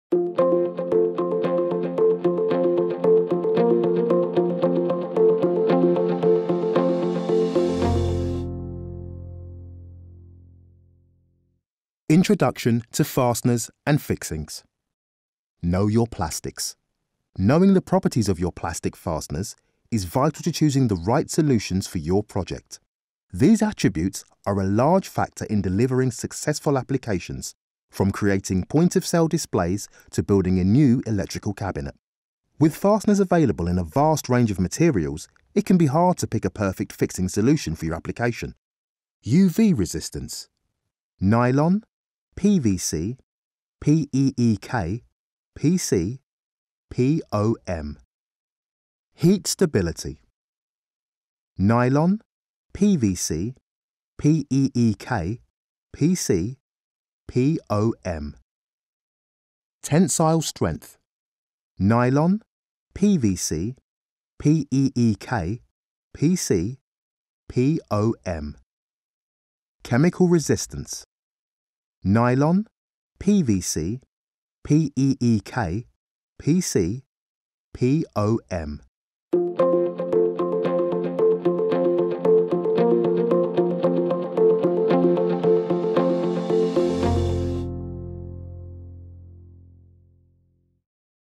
Fasteners & Fixings Materials | Essentra Components
With so many fasteners out there, how do you know which one is the most cost effective while giving you the performance you need?
It comes down to understanding the fastener types and materials. This visual guide is designed to give you an introduction to commonly used materials for fasteners and fixings.
Request a free sample and CAD drawing and explore how we can support your projects with our PCB hardware solutions.